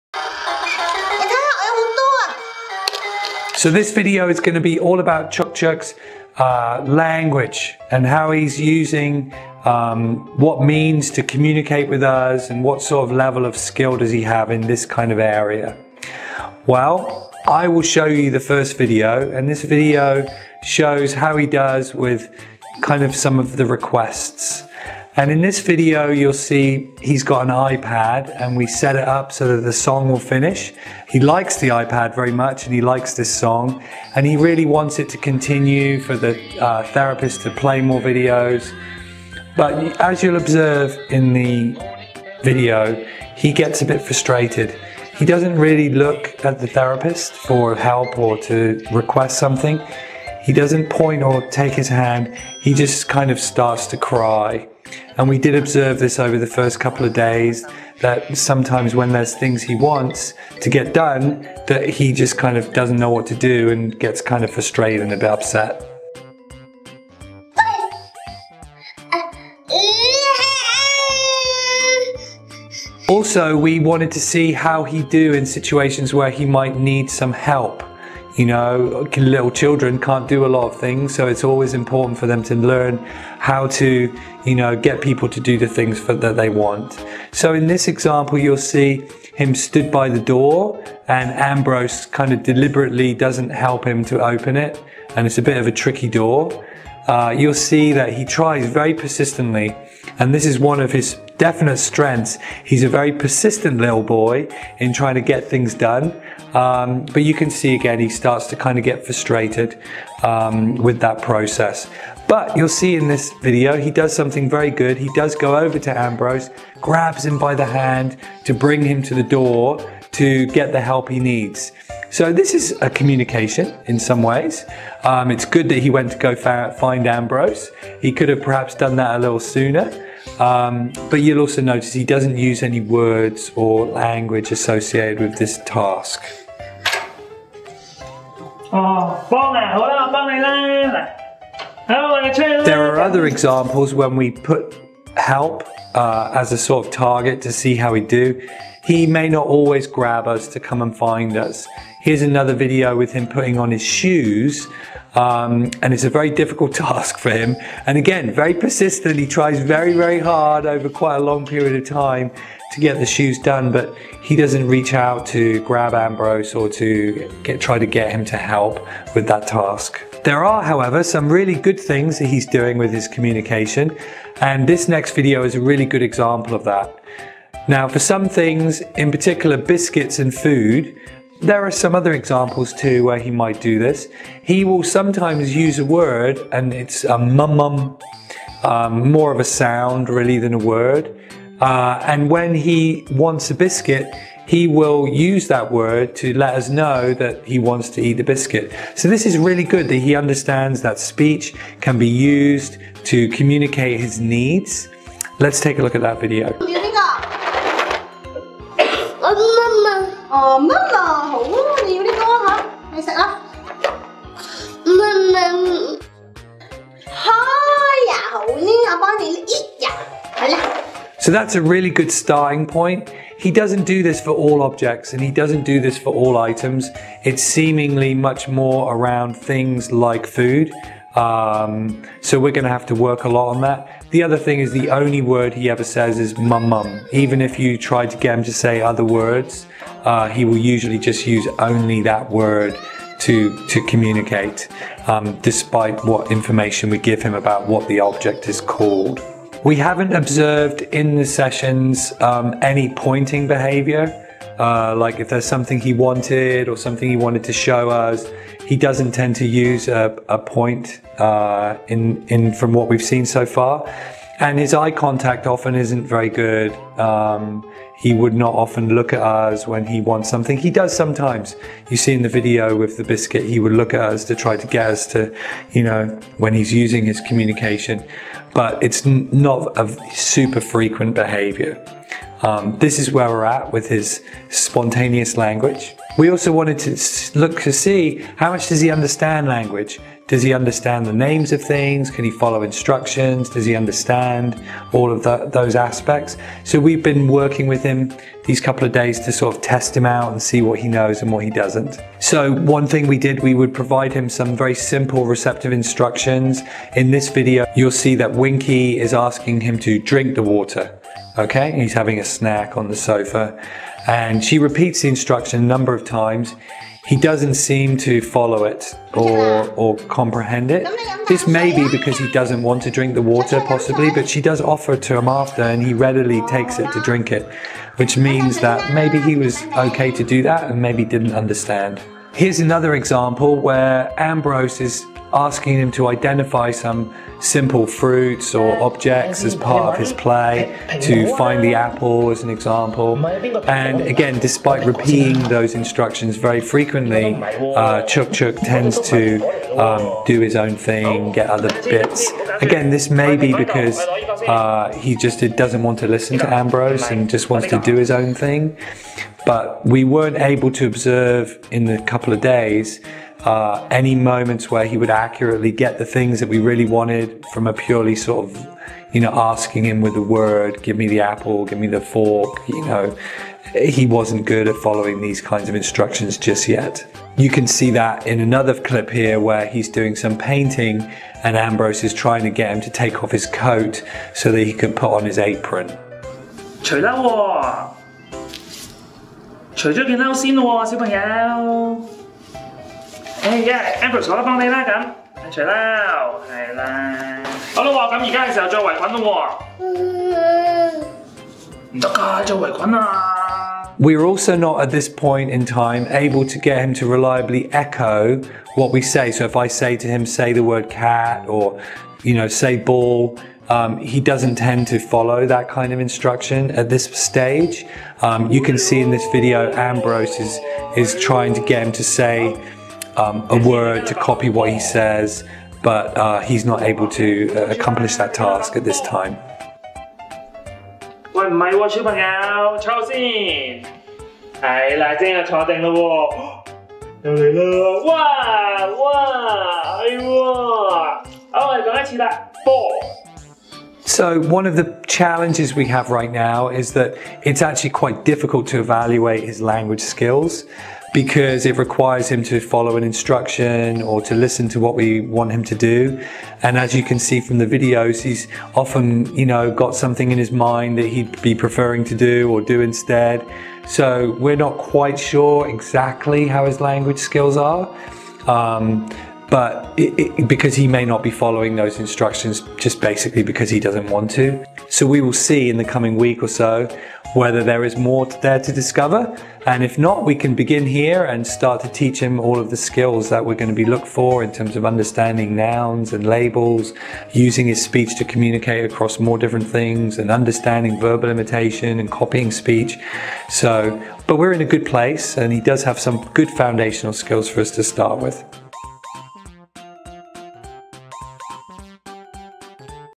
АВА-терапия||Autism Partnership HK|Прогресс Чук Чука|1-ая Неделя.Оценка навыков речи и коммуникации.
Тоби Маунтджой рассказывает о том, каким образом специалисты Autism Partnership HK оценивали уровень коммуникативных навыков Чук Чука, и о выбранных обучающих целях:
- использовать слова и звуки для запросов;
- использовать невербальные средства коммуникации для запросов;
- устанавливать зрительный контакт;
- понимать инструкции на слух
- и многое другое.